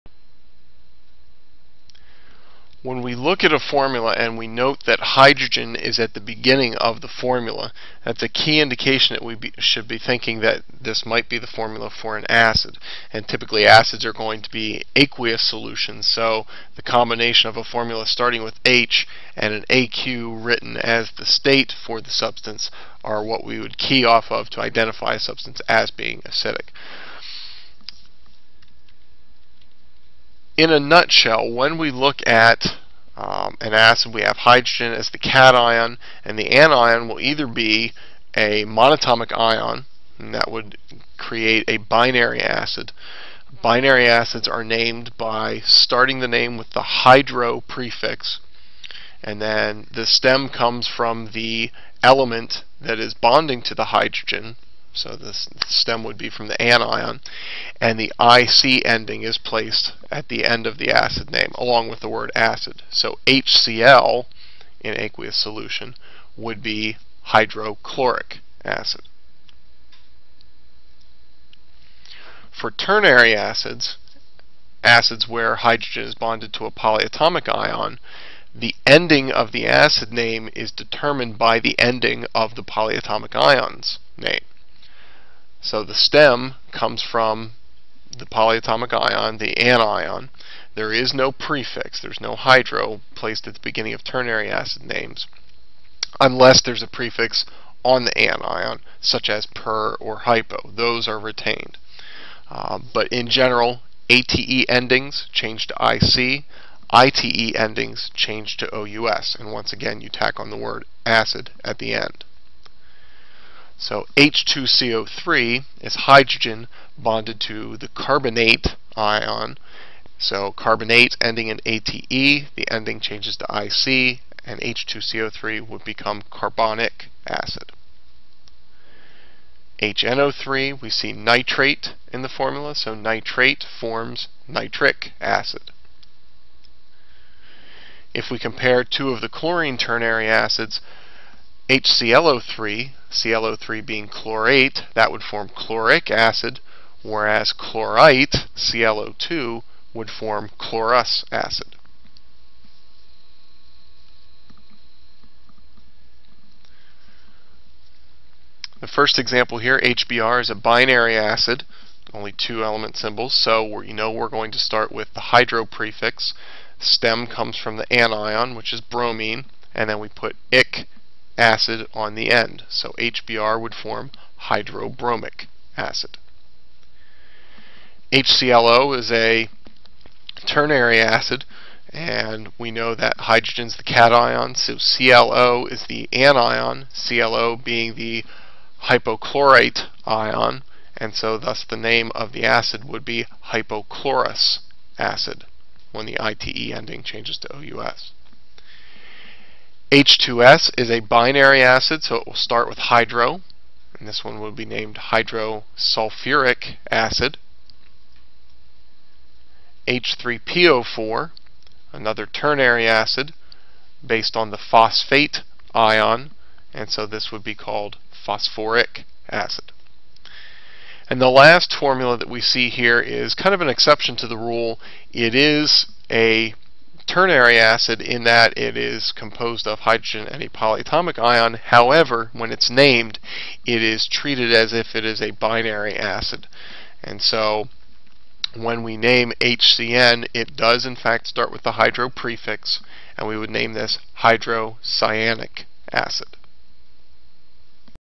Naming Binary & Ternary Acid Formulas (EisleyChem)
This lesson describes the naming conventions used for both binary & ternary (oxyacid) acid formulas.